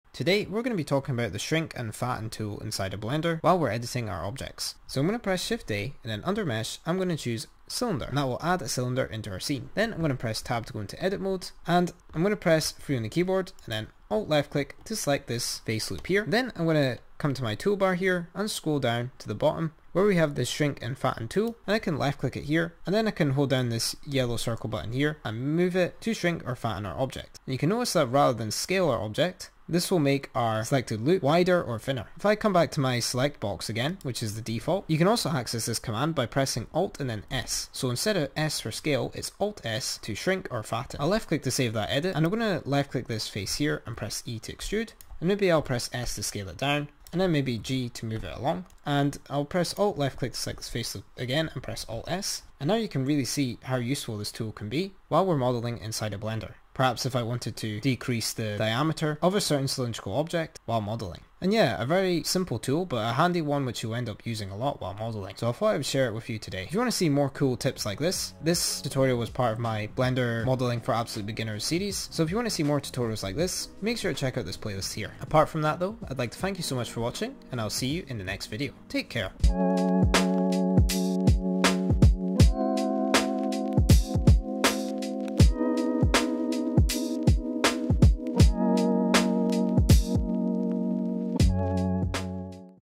How to change an Objects Thickness in Blender 4.2!
Today I cover all you need to know about shrink and fatten tools in blender!

Timestamps:

00:00 Blender Tutorial Overview
00:16 Blender Shrink and Fatten Tool (Edit the thickness)
00:34 Blender Shrink and Fatten Hotkey/Shortcut